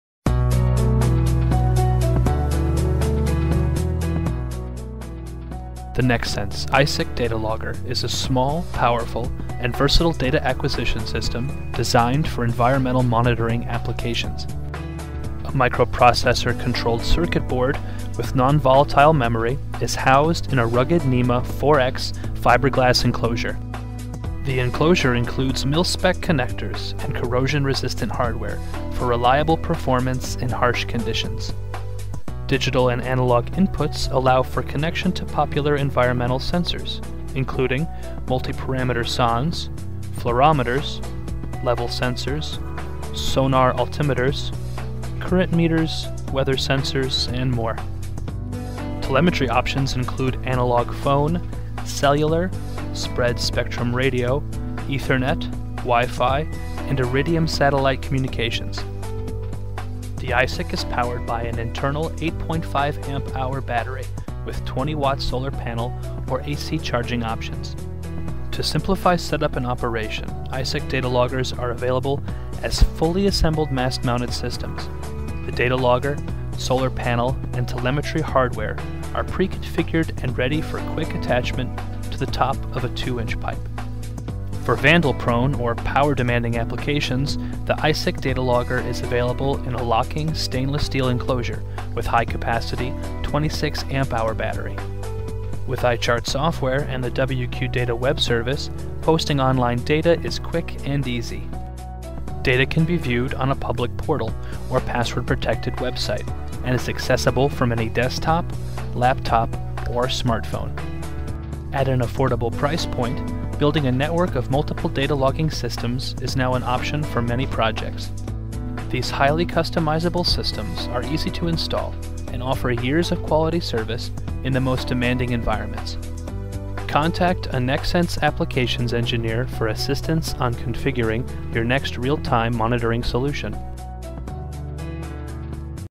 NexSens iSIC Data Logger
The NexSens iSIC (Intelligent Sensor Interface & Control) is the latest in data logging technology. Specifically designed for remote environmental monitoring applications, the iSIC offers superior data acquisition performance in extreme conditions. NexSens customers expect reliability, and the iSIC data logger delivers unmatched sensor interface and data collection with options for landline phone, cellular, radio, Ethernet, WI-FI, and satellite telemetry. Each iSIC data logger simultaneously supports 8 analog inputs & 4 digital inputs for multi-sensor data logging capability.